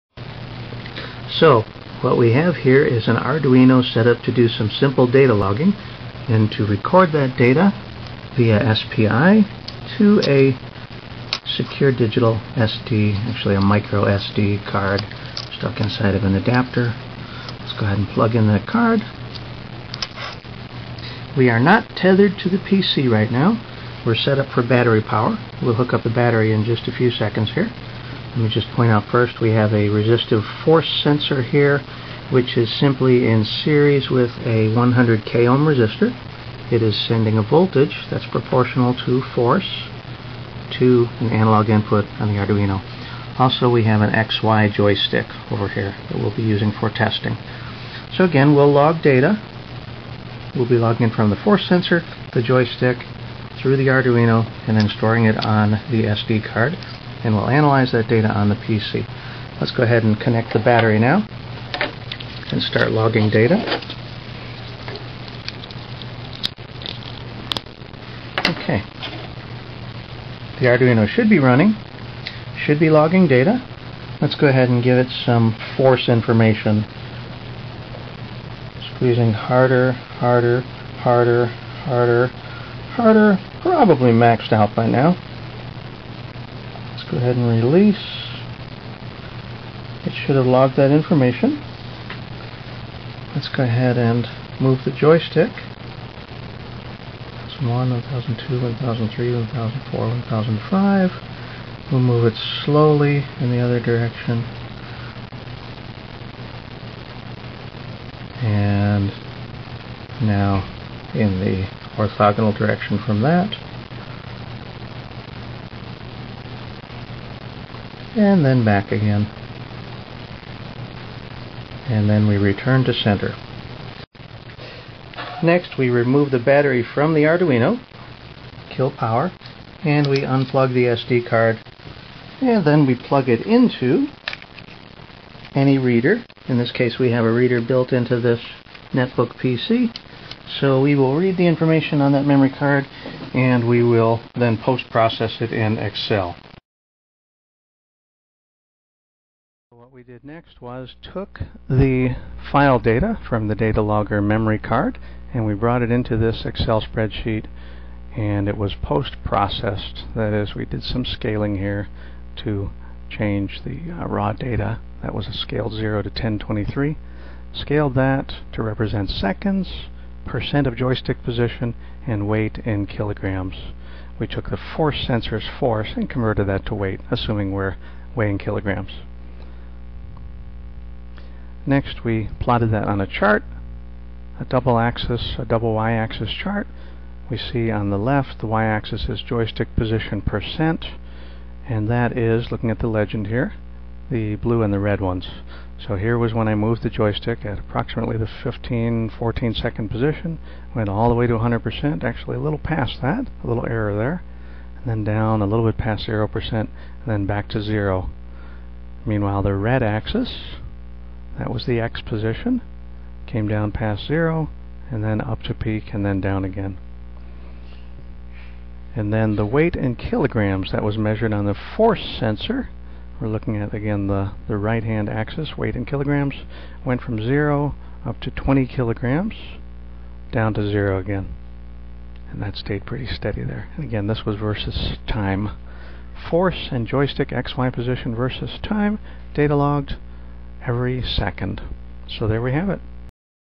Arduino Logging Force and Joystick Position to an SD Memory Card
An Arduino Duemilanove is connected to a Sensor Shield purchased from Yourduino.  A 2-pot joystick is attached as well as a breadboarded resistive force sensor (FSR).  They go to inputs A0-A2.  The SD card is plugged into an SD card reader-writer, also from Yourduino.  I didn't have the right interface shield for the Arduino, so the two are interconnected by jumpers.  Klugy, but it works.  The Arduino is running a simple program that contains code from the free SD examples online as well as the Time library example from the Arduino Playground online.